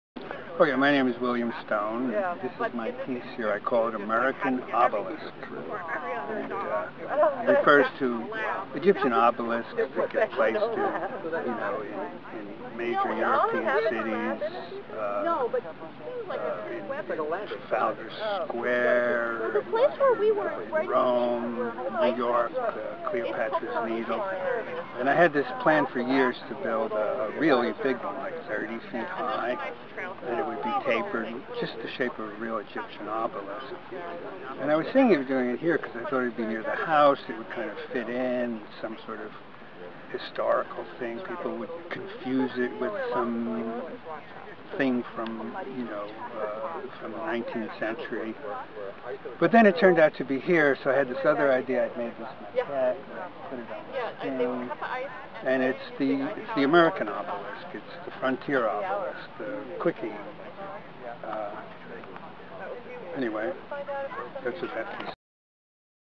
Friends of Clermont Potential Fields artist: William Stone
Meet an artist from the Clermont State Historic Site's Potential Fields Exhibition!